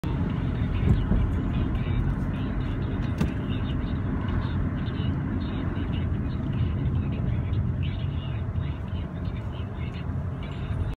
Elizabeth, NJ (take 669): Dodge Durango unyielding to oncoming traffic and a front-end drive down...
Dodge Durango unyielding to oncoming traffic and a front-end drive down please check out my twitter feed @thcsoy